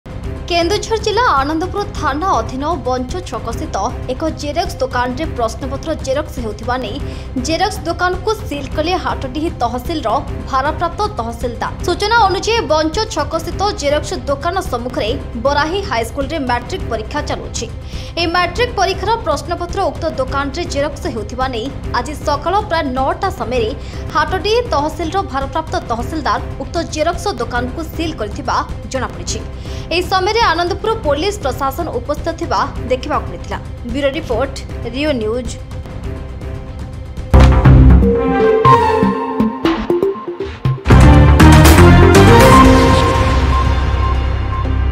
ମାଟ୍ରିକ ପ୍ରଶ୍ନପତ୍ର ଜେରକ୍ସ କରୁଥିବା ନେଇ ବଞ୍ଚୋ ଛକର ଜେରକ୍ସ ଦୋକାନକୁ ସିଲ କଲେ ତହସିଲଦାର..@reonews
REO NEWS brings all the latest headlines and news stories to satisfy the demands of high-rise favorites of Odisha audiences.